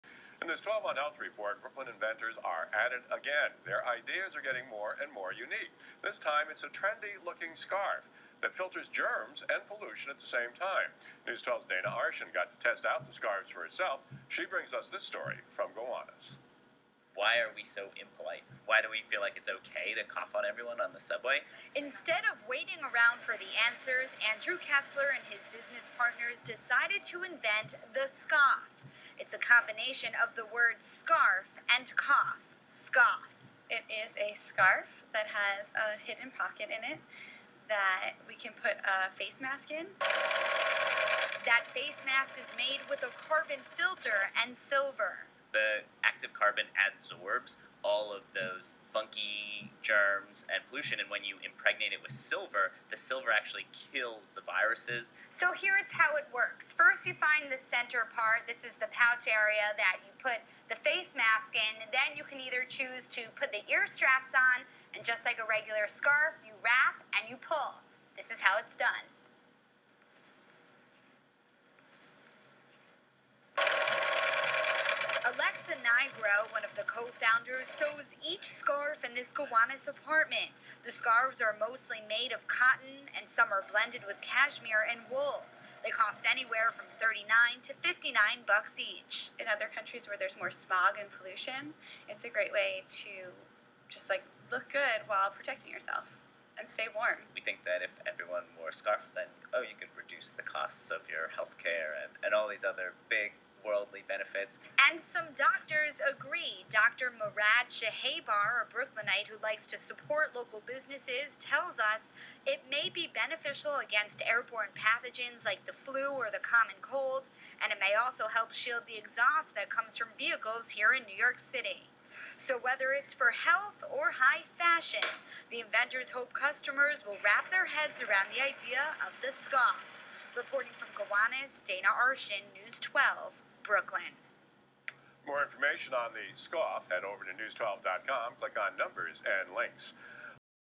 Brooklyn inventors create the 'scough,' a germ-fighting scarf
A group of Brooklyn inventors have designed a special scarf they say filters out germs and pollution.

The scarf, or "scough," features a hidden pocket with a face mask that contains a carbon filter and silver. The carbon filter adsorbs germs and pollution and the silver kills viruses.